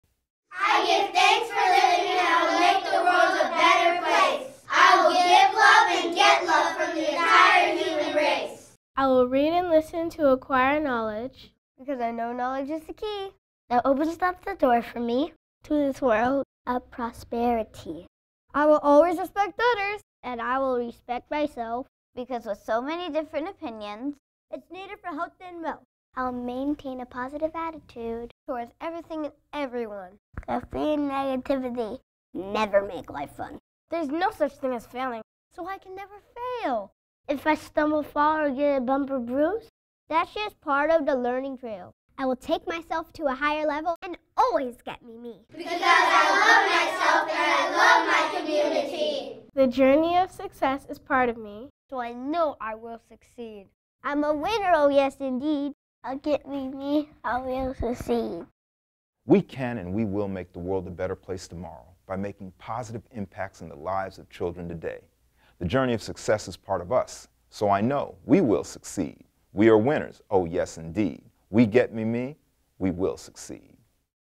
LOVE 101 DAILY PLEDGE
To "Get Me Me" means to LOVE yourself, so you can LOVE others, which is the foundation of LOVE 101and our global mission to share LOVE with everyone, everyday, everywhere.

The LOVE 101 Daily Pledge was created by Mark Saint Juste, while teach his students at Deerfield Park Elementary School in Broward County, Florida, USA.